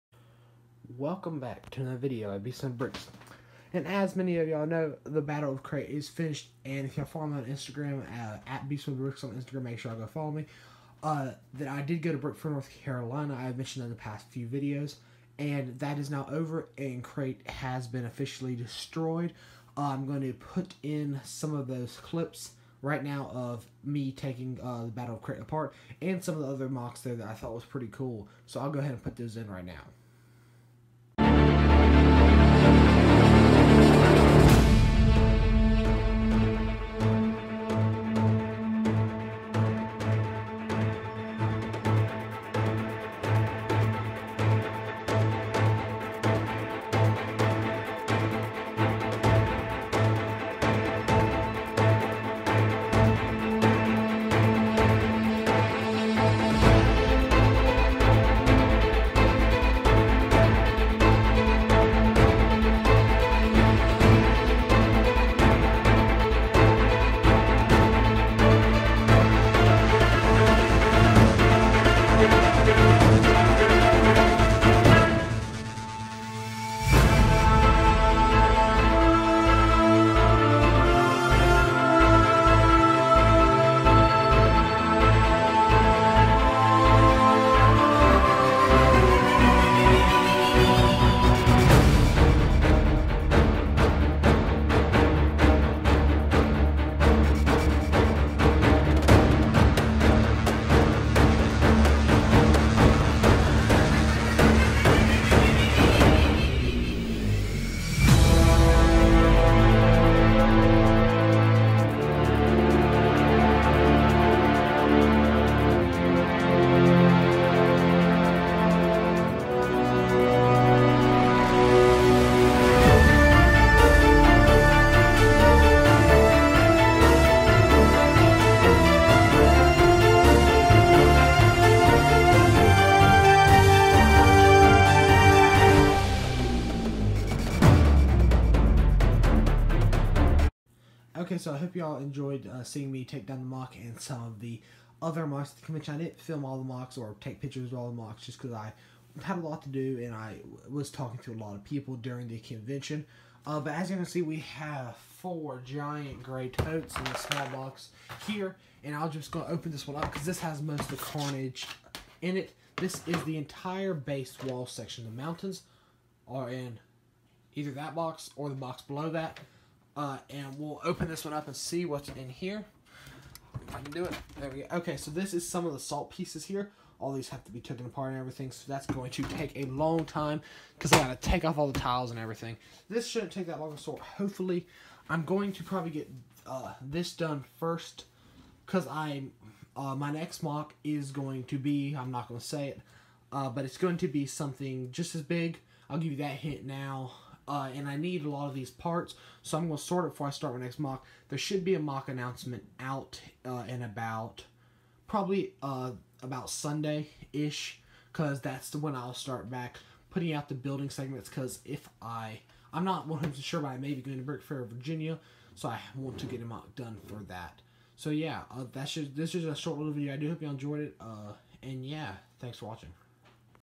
BrickFair North Carolina
I had a great time at Brickfair North Carolina and saw a lot of great creation but it is now time to start taking Crait apart.